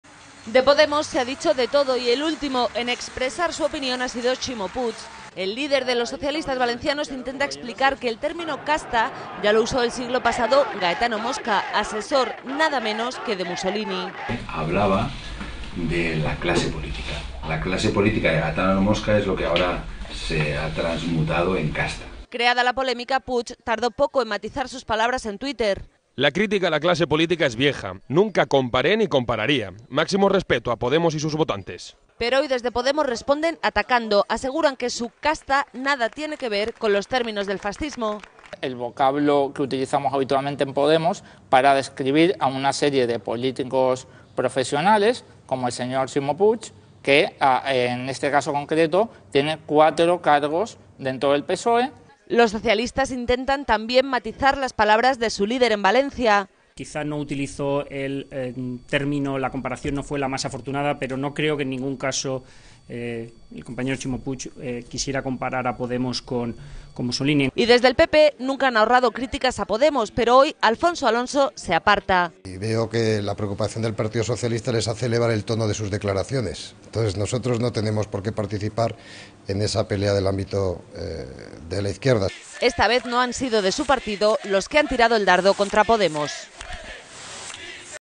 El PSOE compara el discurso de Podemos con el de Mussolini
Ximo Puig, secretario de Regeneración Democrática del PSOE, asegura que el discurso de la formación liderada por Pablo Iglesias, Podemos, en contra de la “casta política” se basa en argumentos “viejos” y similares a los preceptos de Gaetano Mosca, un intelectual italiano al que “acudió con frecuencia el dictador Benito Mussolini”.